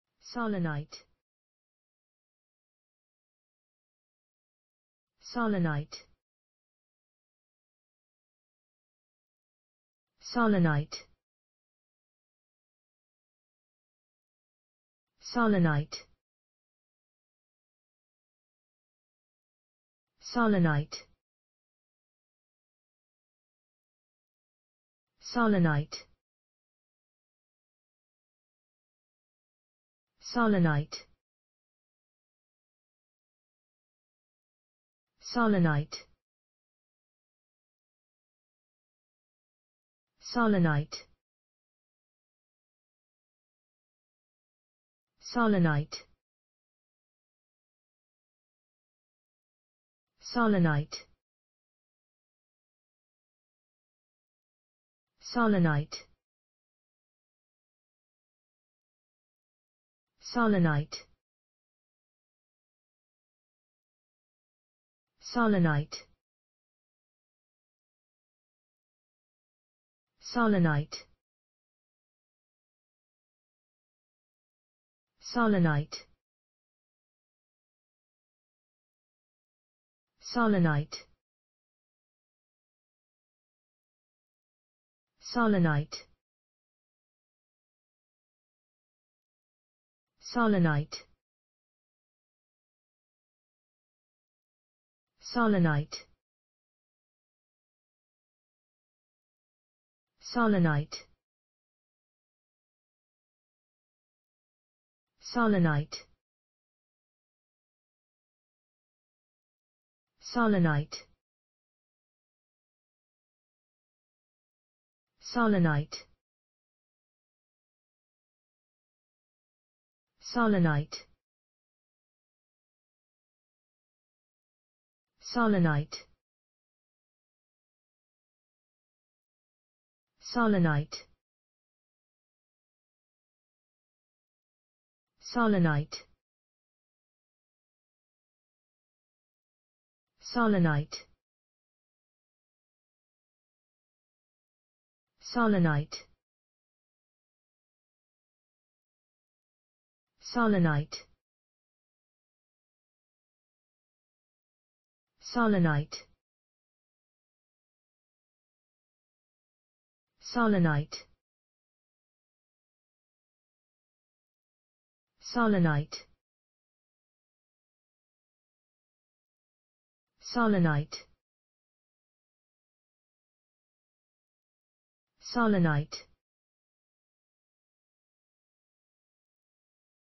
sahlinite. How to pronounce the english word sahlinite .Start with S. Learn from me.
Follow me!Learn from me how to pronounce the english word sahlinite.This video shows you how to pronounce the English word sahlinite .